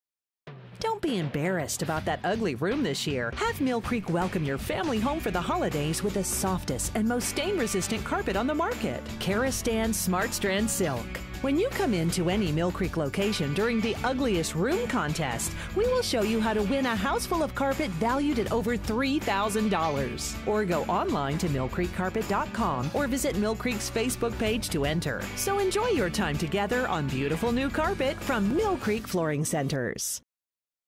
Mill Creek's Ugly Room Contest
Your Ugly Room Could Win you $3,300 worth of Carpet!
You could win a gift the whole family will enjoy for years to come. By entering a photo of your ugly room into our Ugly Room Contest you could win $3,300 worth of Karastan SmartStrand Carpet. Karastan SmartStrand Carpet is the ONLY carpet with built-in lifetime stain and soil resistance that will NEVER wear or wash off. Have peace of mind with SmartStrand's superior durability and comfort.

Need carpet now but still want to win it for free?
Come in to Mill Creek and let our sales team help you find the perfect Karastan SmartStrand Silk carpet for your home. If you win the Ugly Room Contest, we will reimburse your previous purchase up to $3,300. Hurry! Contest ends December 15, 2012.